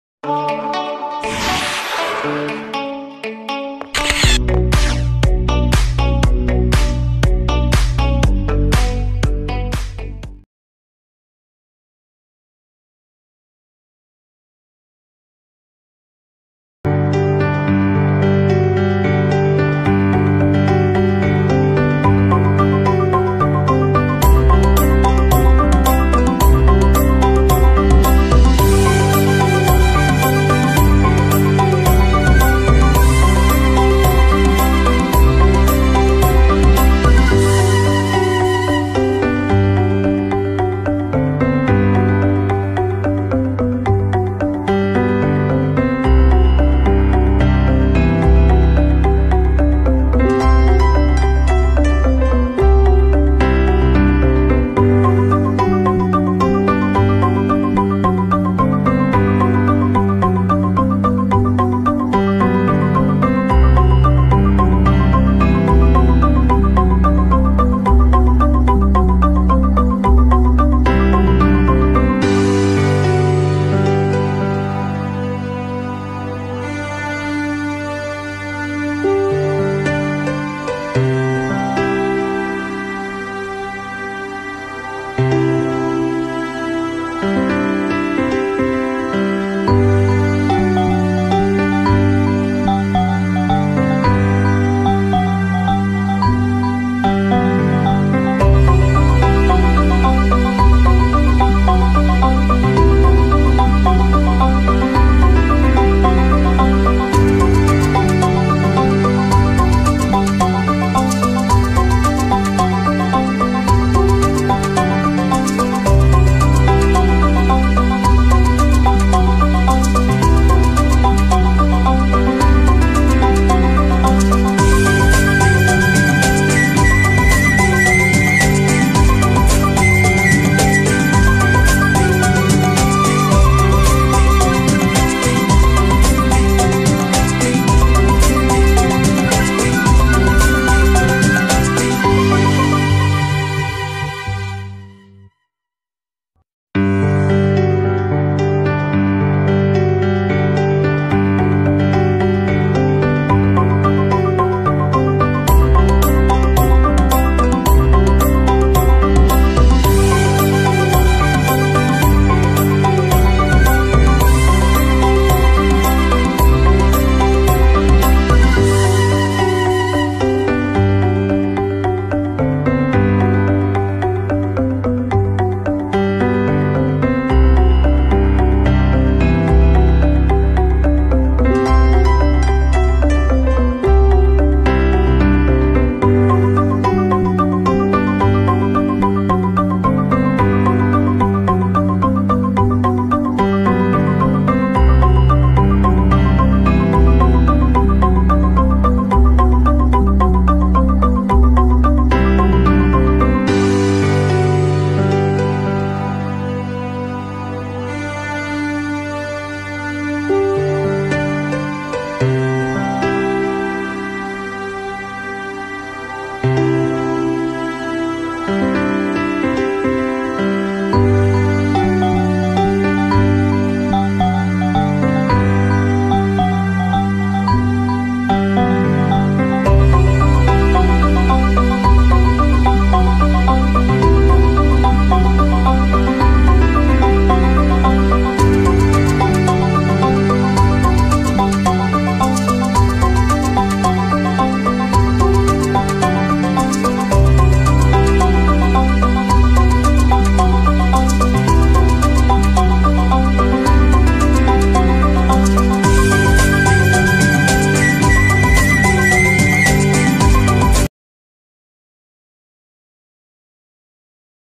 Panorama Serang Pratin Purbalingga Jawa Tengah
Kabupaten Purbalingga Jawa Tengah Terus Berbenah dengan Keindahan Alamnya Di sekitar kaki Gunung Slamet...
Ya Desa Serang Pratin adalah salah satunya dengan andalan Kebun buah Sayur mayur Dan Taman Bunga Yg begitu Romantic
Dataran Gunung Slamet Meliputi 5 Kabupaten
Di Selatan Ada Kab. Banyumas
Di Utara Ada Kab Pemalang dan Tegal
Di Barat Ada Kab Brebes
Dan Di Timur Meliputi Kab Purbalingga

wisata desa serang pratin purbalingga,